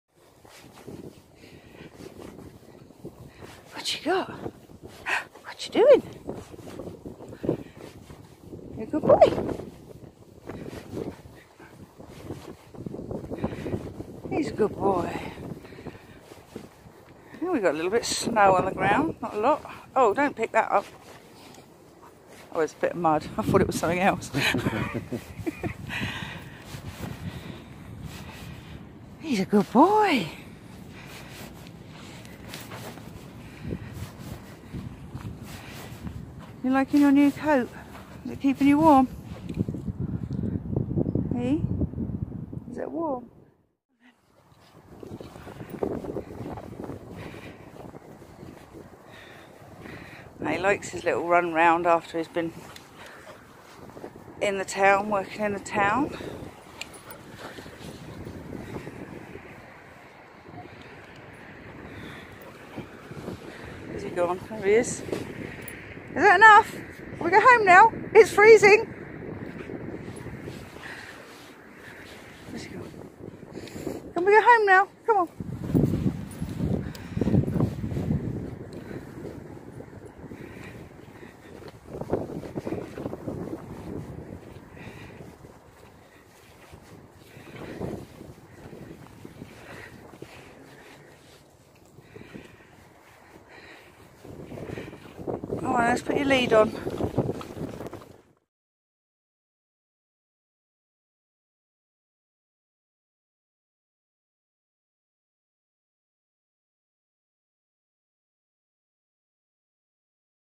A Small Amount of Snow, ❄️🌨️❄️🌨️
Sadly we never get much snow down here in the South of England, but more is forecast for tonight so we can't wait to see what it's like tomorrow morning, Boris loves playing in the snow 😀